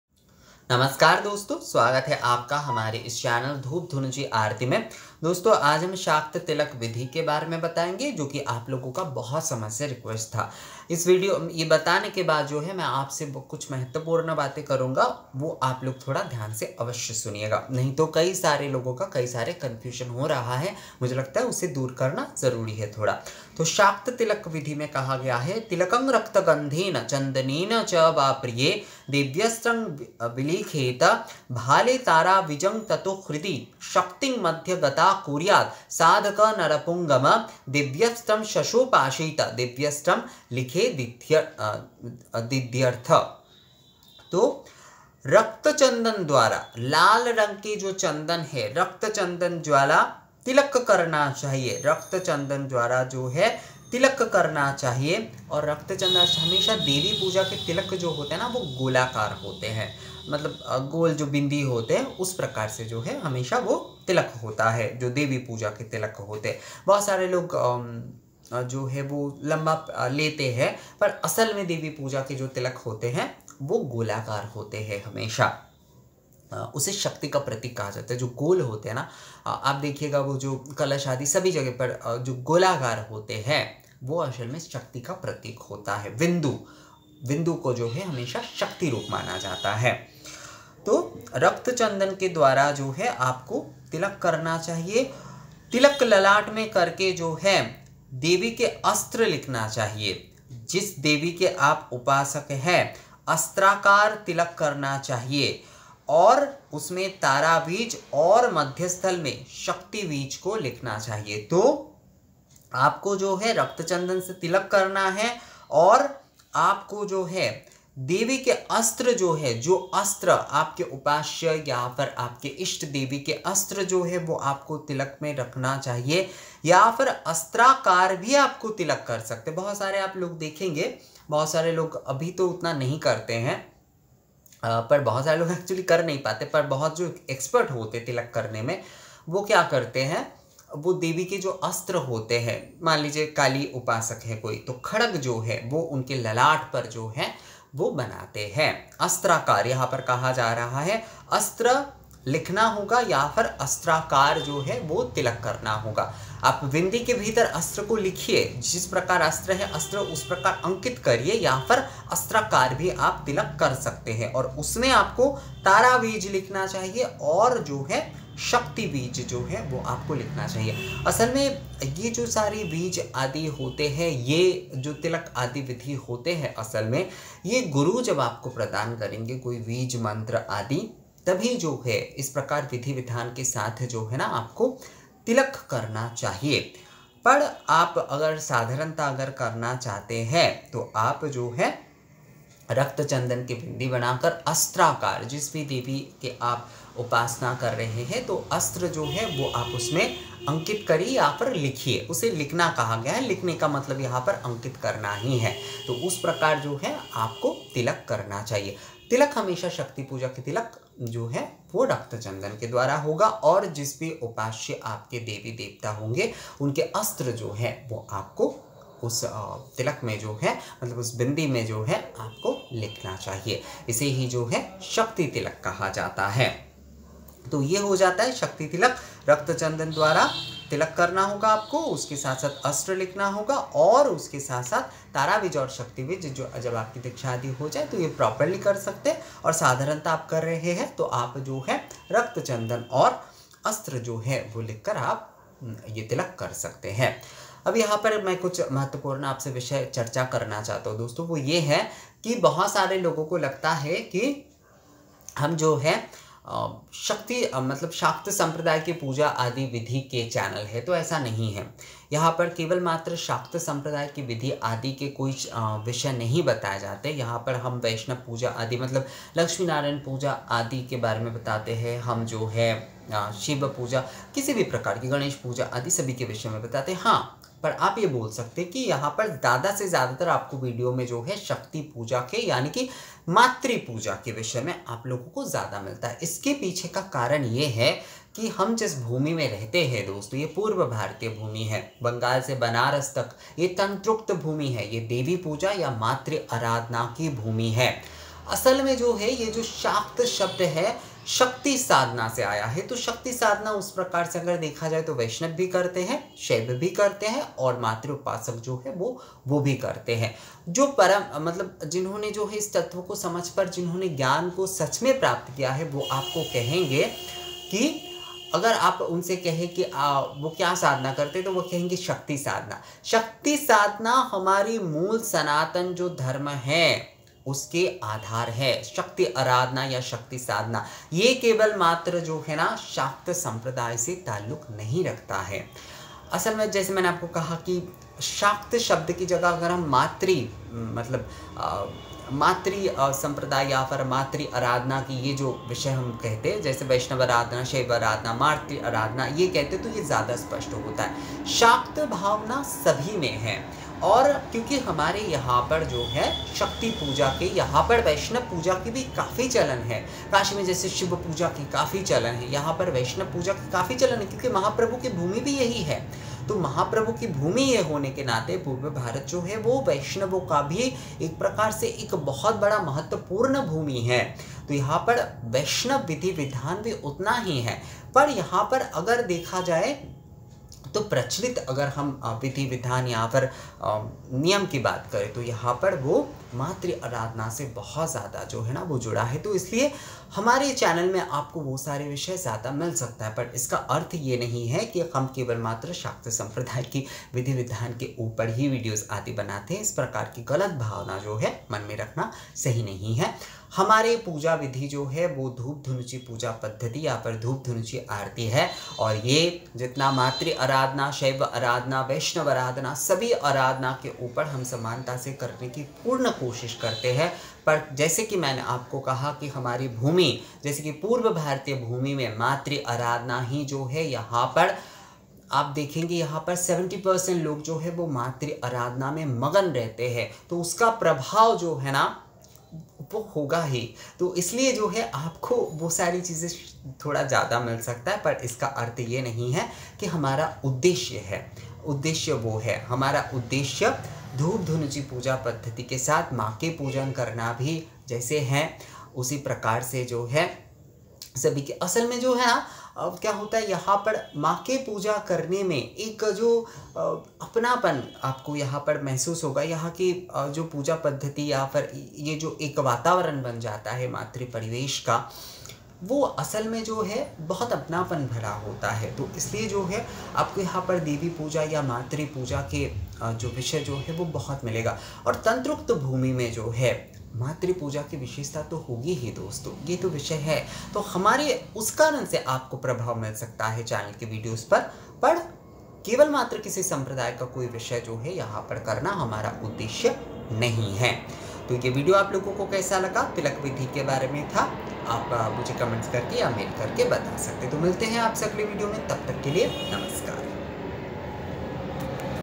शास्त्र अनुसार शाक्त तिलक | Shastra Anusar Shakti Puja Tilak | शाक्त आराधना तिलक धारण
धूप धुनुची आरती

            " ॐ वनस्पति रसो दिव्यो गन्धाढ्यः सुमनोहरः |
               मया निवेदिता भक्त्या धूपोहयं प्रतिगृह्यताम|| "

               "ॐ वनस्पतिरसोद्भूतो गन्धाढ्यः गंध उत्तमः|
                  आघ्रेयः सर्व्वभूतानां धूपोहयं प्रतिगृह्यताम्||"

               " ॐ वनस्पति रसो दिव्यो गन्धर्वा सुरभोजनः|

दिव्य वनस्पति रस से समृद्ध यह सुगन्धित धूप जो सर्वदेव के आघ्राण के लिए उत्तम है। में भक्ति भाव से वही धूप आपको प्रदान करता हु कृपया इसे स्वीकार करें।

धुनुची धूप : धूप प्रज्वलन के पीतल पात्र को धुनुची या धूपचि कहा जाता है।  इसे आजकल धूपदान या धूपदानी भी कहते हैं। गोबर के कंडे या नारियल के छिलके से इसे राल गुग्गुल घी या अष्टांग धूप से जलाया जाता है। धुनुची धूप को हवन धूप और यज्ञ धूप भी कहा जाता है। इसलिए इसे धूपाग्नि भी कहा जाता है।